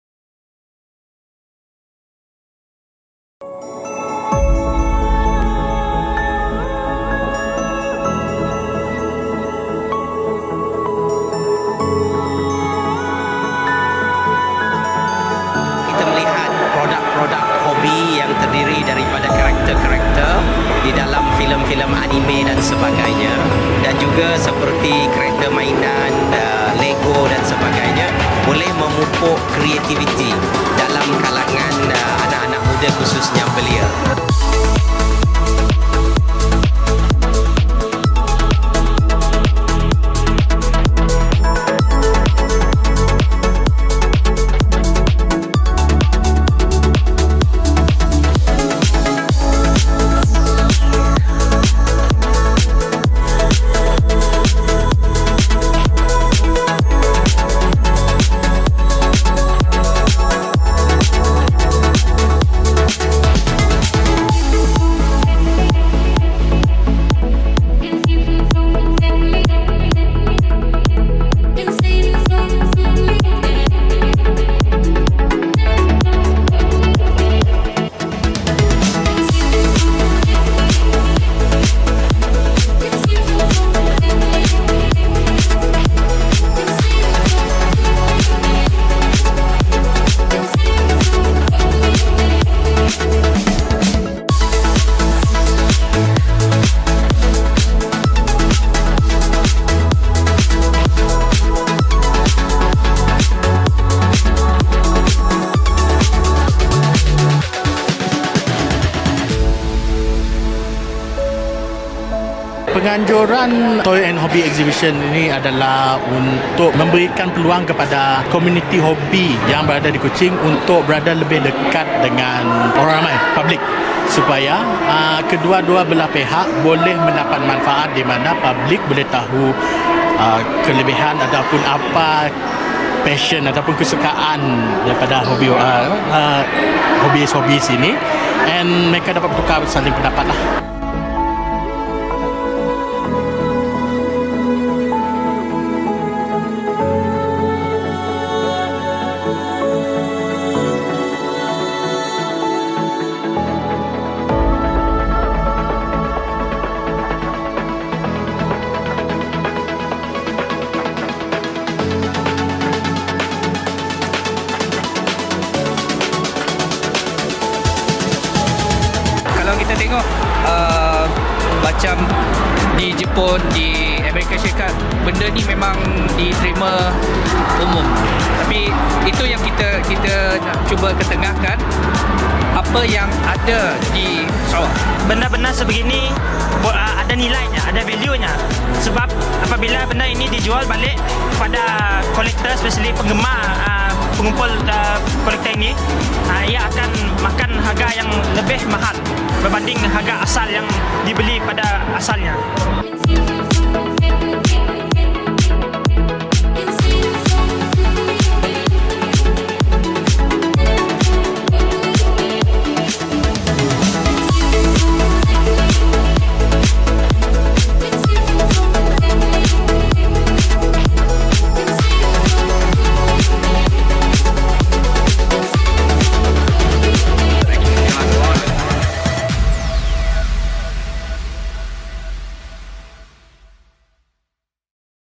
Toys & Hobby Exhibition 2017
Event : Toys & Hobby Exhibition 2017
Venue : Sarawak State Library, Kuching, Sarawak, MALAYSIA
Date : 13-14 May 2017